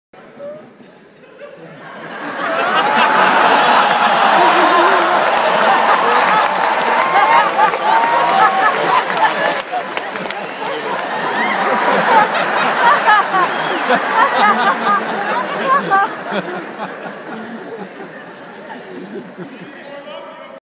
NOVAK DJOKOVIC
Novak Djokovic comes down to play doubles after a 'request' from McEnroe